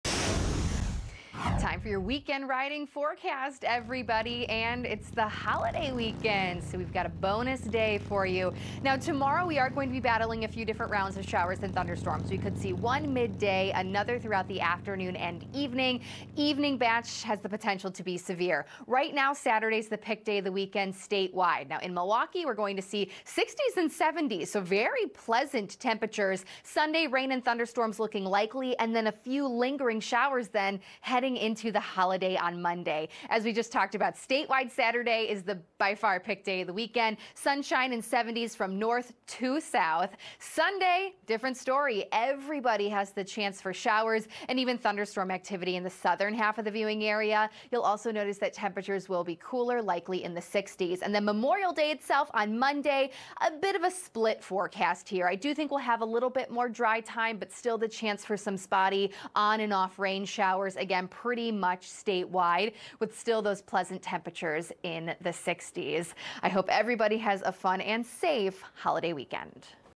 Weekend Riding Forecast (May 24-27) | FOX6 News Milwaukee
Soak up the sun on Saturday, because after that there is a good chance for rain to get in the way of your holiday weekend motorcycle ride. FOX6's Stephanie Barichello has your Weekend Riding Forecast for May 24-27.